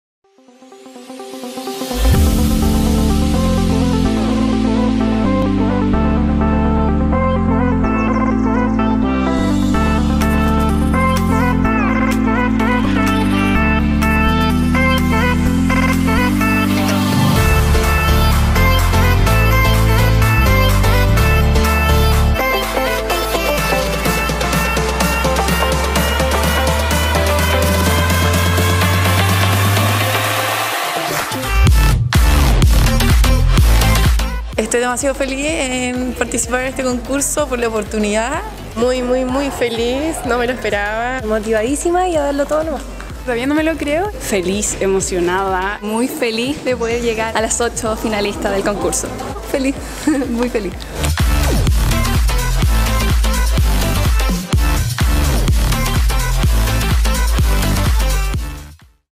VIDEO CASTING MISS DERBY 2019
El viernes 23 de noviembre del 2018 se realizó el casting que permitió la elección de las 8 finalistas entre quienes se escogió la representante Miss Derby 2019.

Fue una agradable tarde en donde 24 candidatas llegaron hasta el Valparaíso Sporting, y demostraron una serie de cualidades que llevaron a que la selección de las 8 jóvenes que pasaron a la segunda fase, fue una difícil tarea.

El jurado encargado de la selección estuvo compuesto por Mauricio Maurel y Hernán Robles del Valparaíso Sporting, Cristián Dazzarola, de la Ilustre Municipalidad de Viña del Mar, Andrés Campusano, representante de Renault, Paola Pellerano, experta en protocolo, Shila Aldunate, dueña de la boutique Di Trevi y Roberto Coloma de la agencia Pamela Díaz Caro.

Las 8 finalistas fueron: Constanza Mellado, Constanza Schmith, Daniela Ramírez, Natalia Orellana, Paula Godoy, Romina Pancetti, Sofía Finsterbusch y Sofía Tornquist, quienes presentaron en el desfile de la Gala del Miss Derby, que se llevó a cabo el día 11 de diciembre 2018.